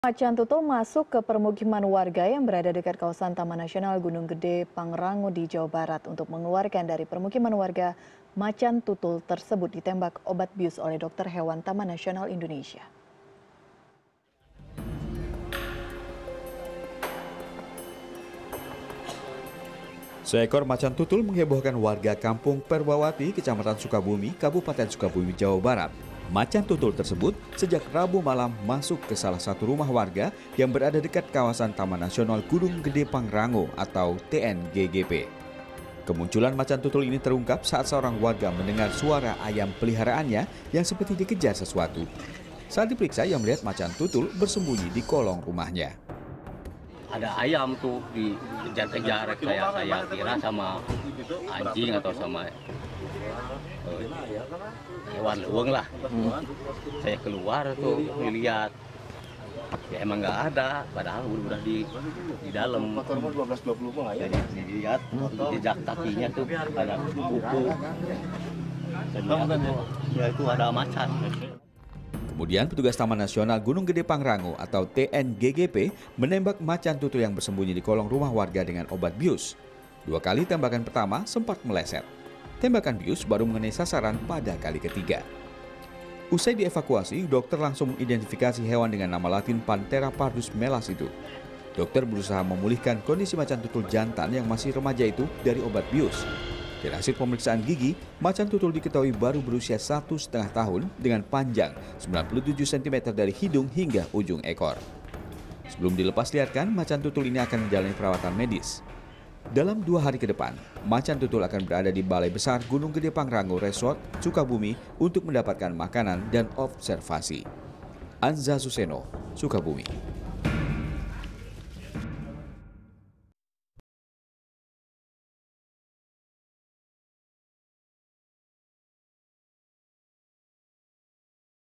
Macan Tutul Masuk Rumah Warga
Seekor macan Tutul masuk ke permukiman warga yang berada dekat kawasan Taman Nasional gunung Gede Pangrango, di Jawa Barat. Untuk mengeluarkan dari permukiman warga, macan Tutul tersebut ditembak obat bius oleh dokter hewan taman nasional Indonesia.

Like & Follow Facebook:
CNN Indonesia

cnnindonesia